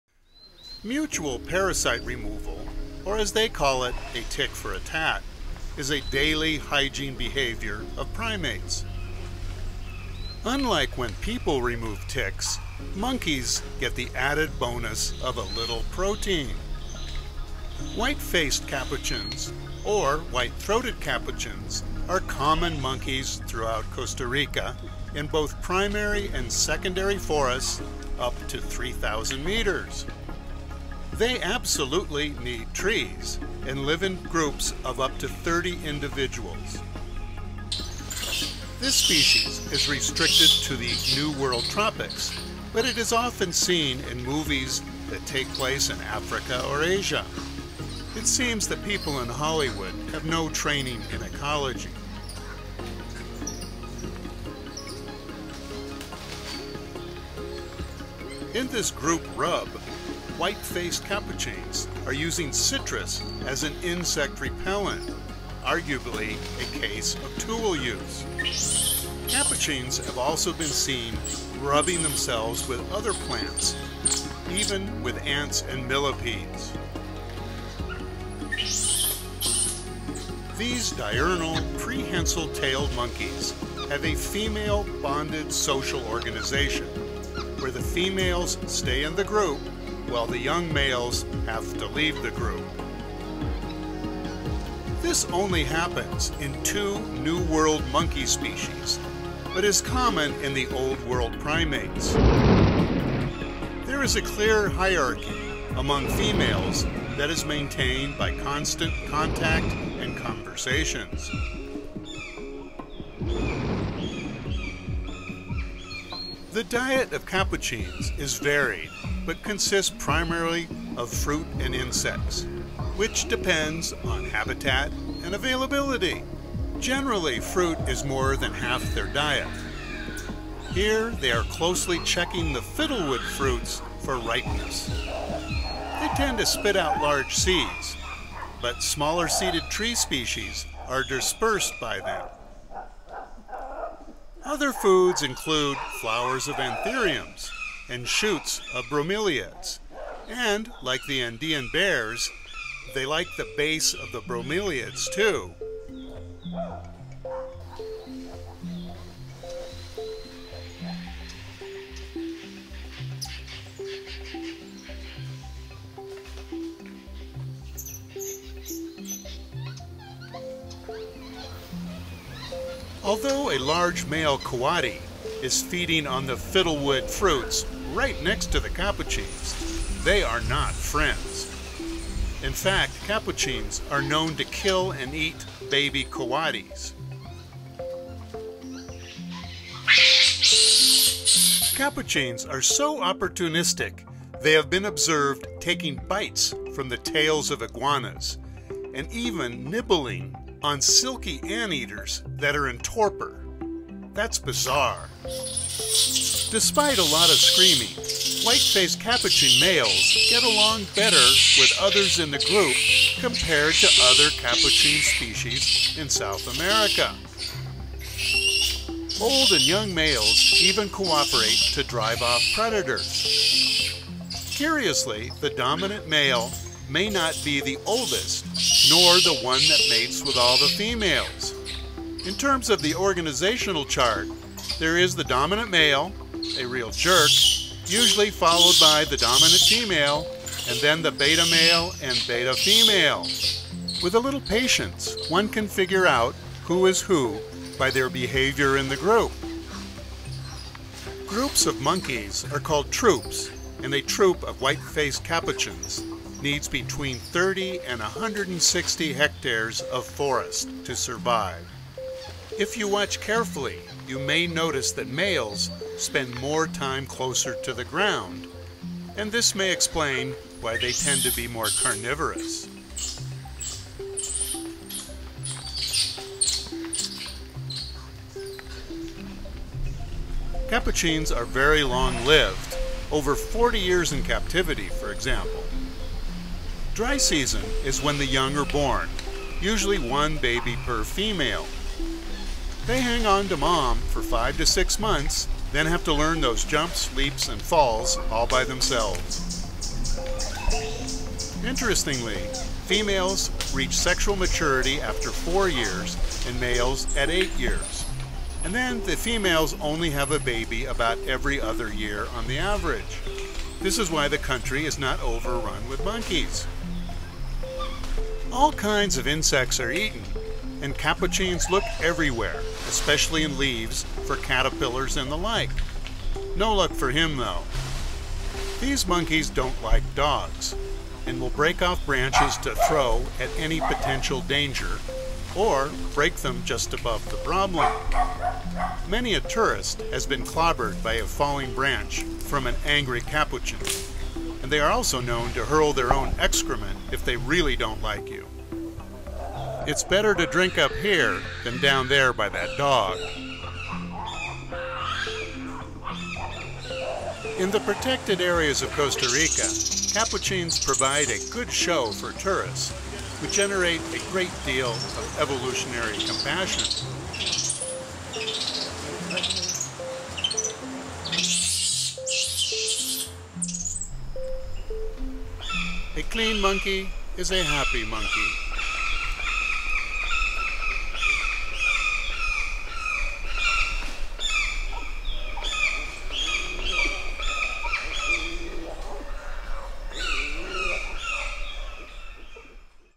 The White-faced Capuchin in Costa Rica
The White-faced Capuchin is a small monkey of Central America and northern South America. It inhabitants both secondary and primary forests from the lowlands to the highlands. It's diet is mostly fruit but it eats plenty of insects and small animals.